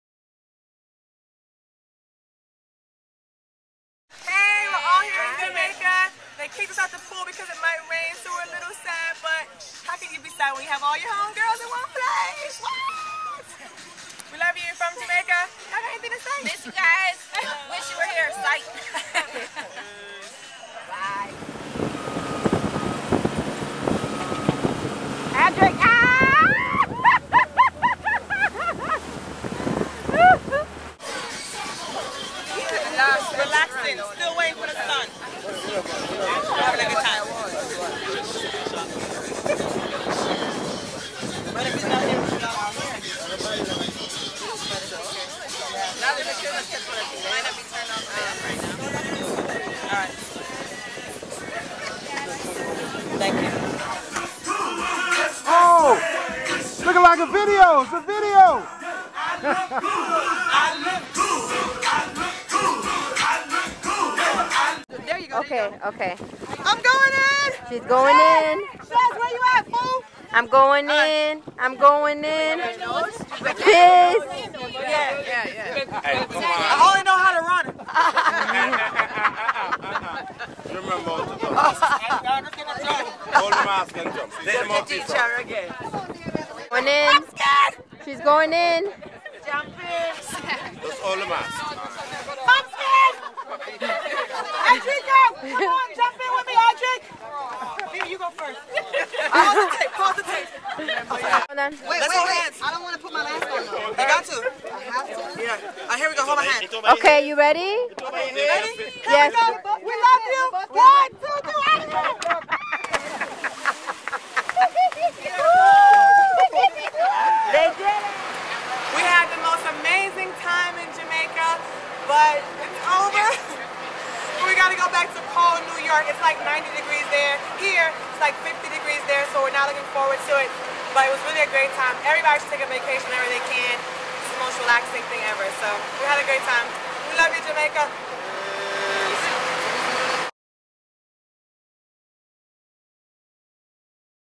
Sanya Vacationing in Jamaica
Watch as my family and friends vacation in Jamaica. We have a blast on the banana boat and on our boat cruise! You don't want to miss this!!!